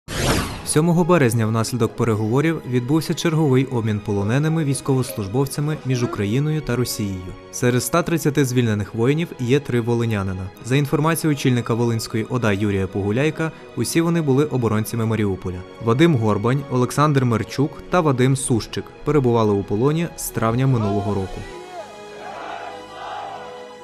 💙💛 З російського полону повернули ТРЬОХ волинян
7 березня внаслідок переговорів відбувся черговий обмін полоненими військовослужбовцями між Україною та росією. Серед 130 звільнених воїнів є три волинянина.